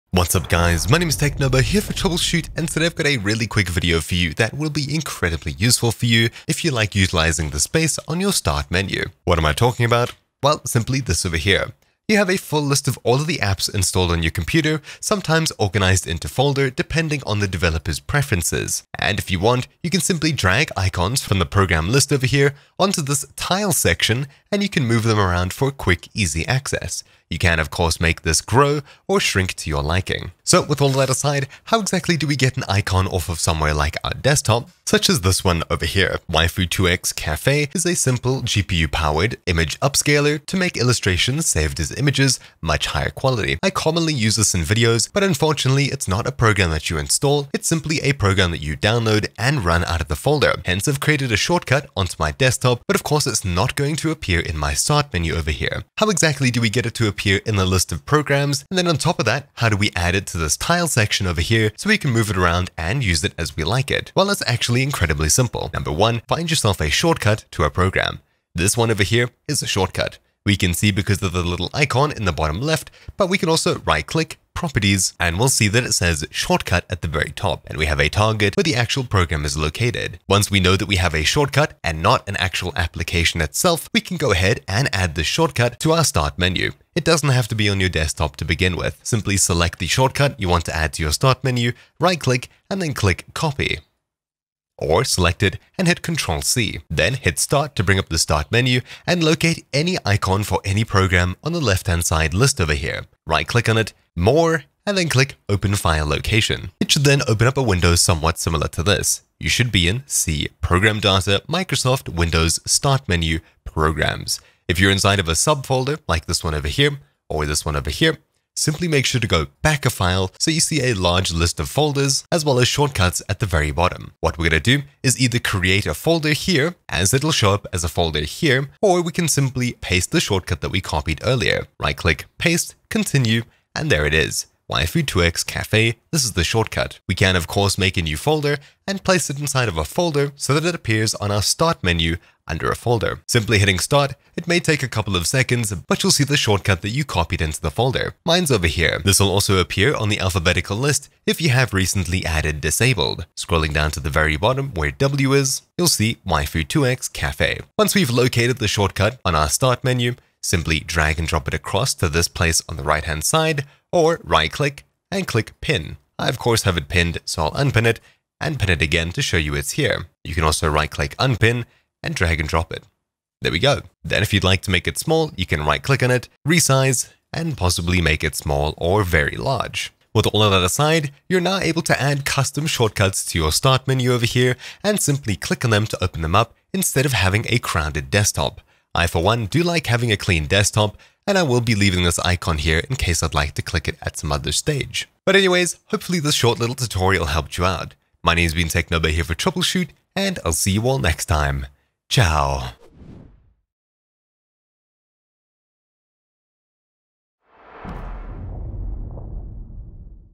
Windows: Custom Shortcuts on Start Menu/Tiles | Guide | Crash Course
Want to add custom files and programs to the Windows Start Menu, or the tiles next to them? Well, it's super simple when you know where to look. This video guides you through all the steps you need to follow to add your own shortcut for a program or file to the start menu.

Timestamps:
0:00 - Explanation
0:55 - Finding a shortcut
1:35 - Finding start menu folder
2:04 - Adding a custom shortcut to Start Menu
2:33 - Adding a custom shortcut to Start Menu Tiles